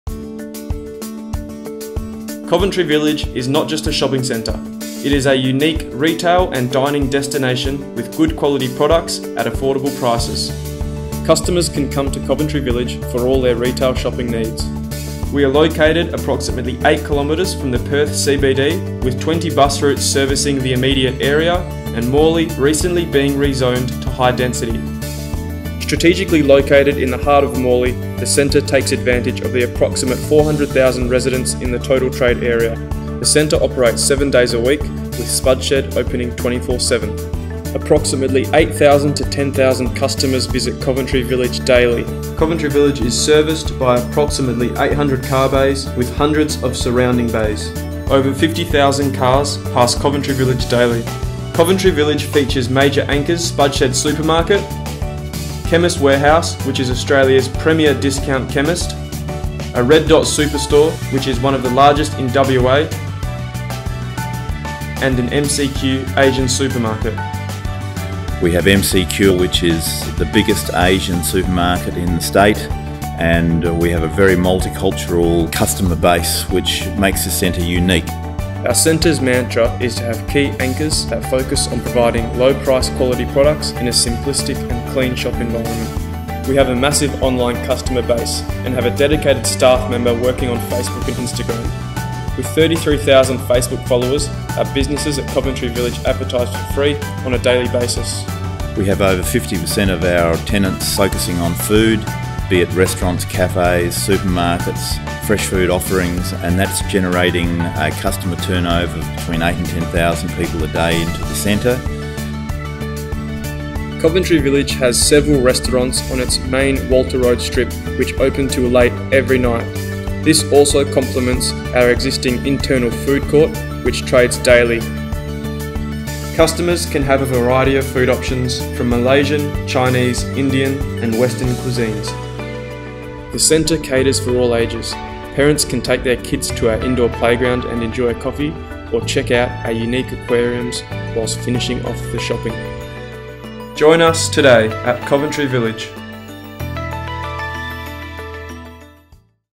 Coventry Village
Coventry Village is not just a shopping centre, it is a unique retail and dining destination with good quality products at affordable prices. Customers can come to Coventry village for all their shopping needs. We are located approx. 8km from Perth CBD with 20 bus routes servicing the immediate area and Morley recently being rezoned to high density. Strategically located in the heart of Morley the centre takes advantage of the approx. 400 000 residents in the total trade area. The centre operates 7 days a week with spud open 24/7. Approx. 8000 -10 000 visors daily, serviced by 800 car bays with hundreds of surrounding bays.

Coventry Village features major anchors Spudshed Supermarket, Chemist Warehouse, Red Dot Superstore which is one of the largest in WA, MCQ Asian supermarket and a Lucky Charm Newsagency. We have a multicultural customer base which makes this centre unique. We have a massive online customer base with a dedicated staff member working on social media, news platforms daily. There is a customer turnover of around 8-10,000 people per day with several restaurant on our Walter Rd strip which operates until late at night. Parents can even take their kids to our unique playground or enjoy our large indoor aquariums.